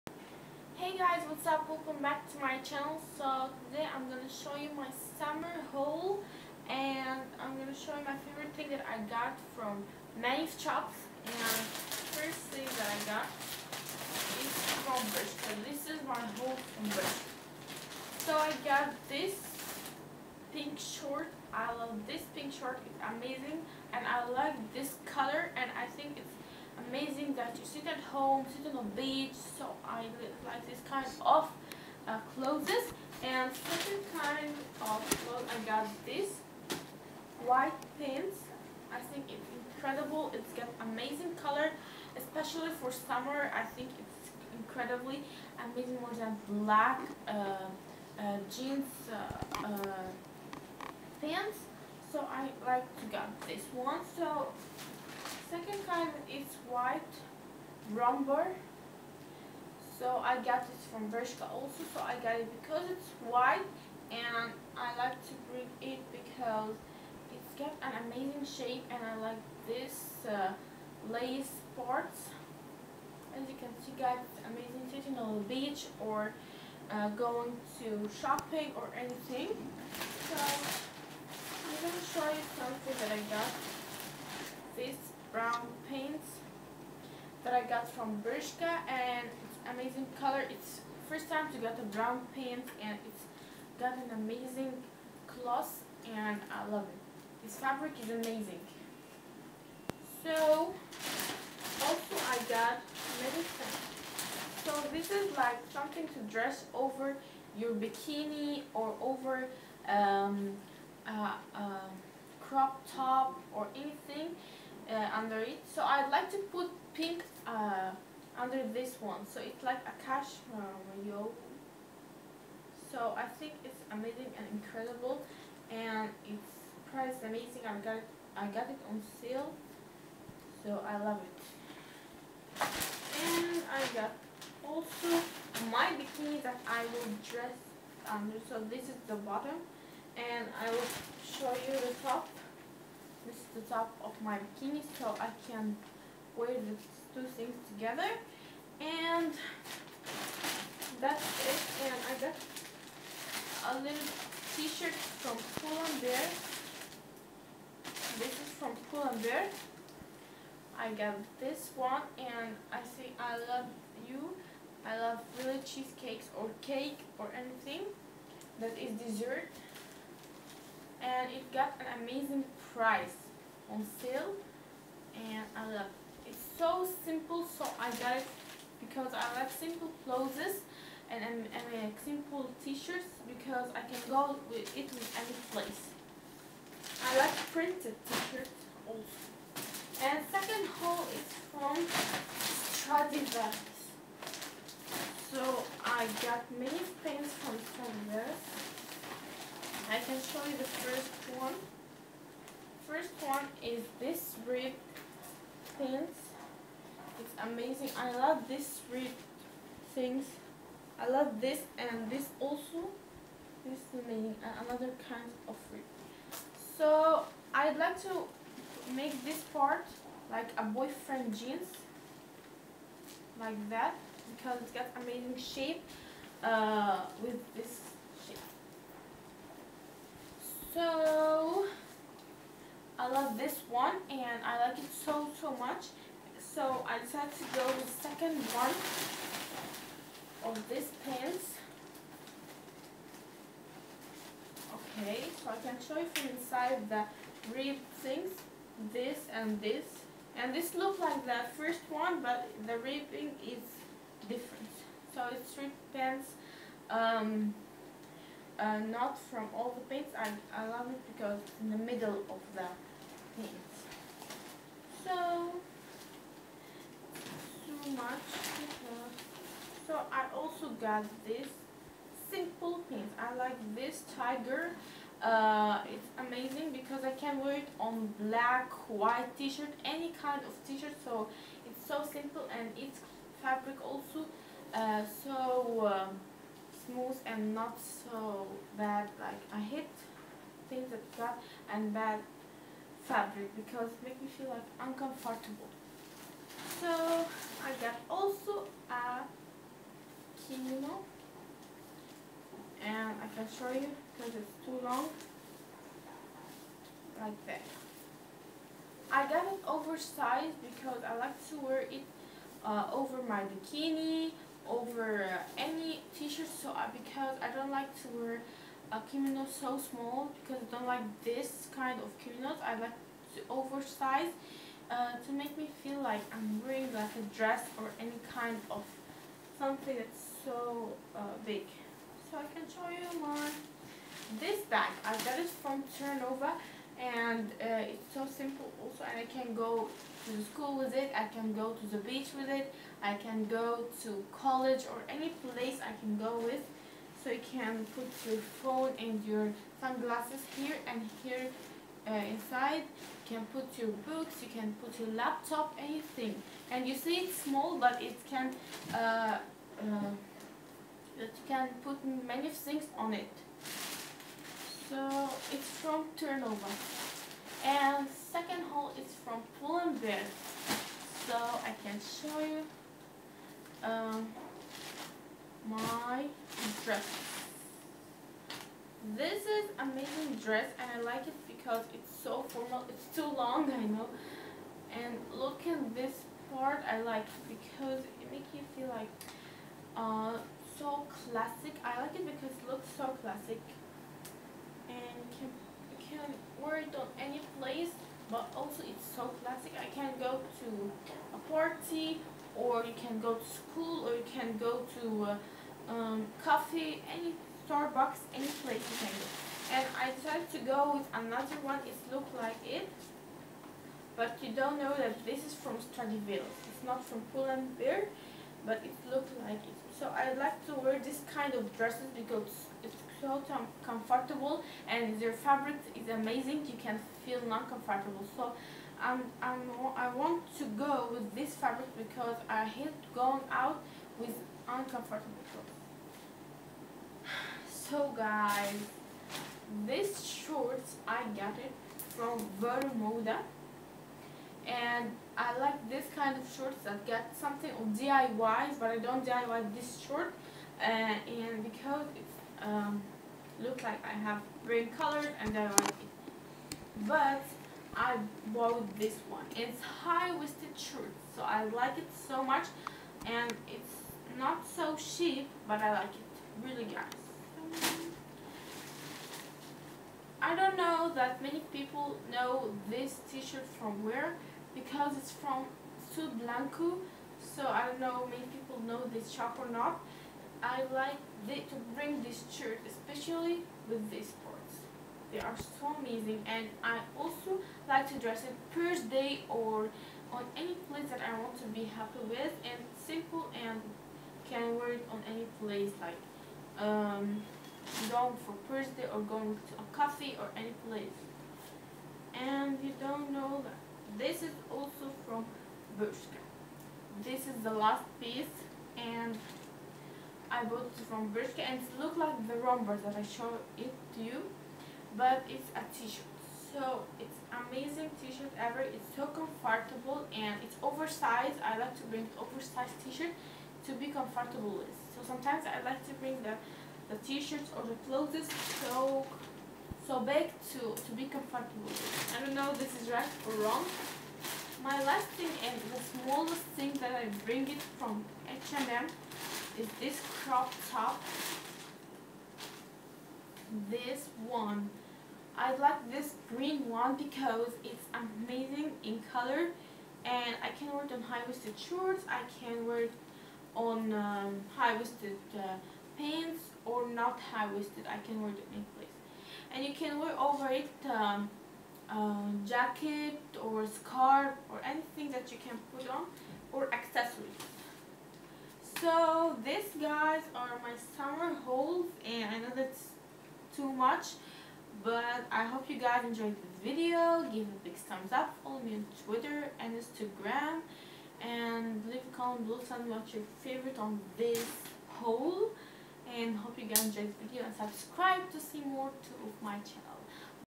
SUMMER HAUL 2016
this video I am gonna show you my summer clothes and am gonna tell you about the brands & you can go for this places & enjoy shopping love you guys the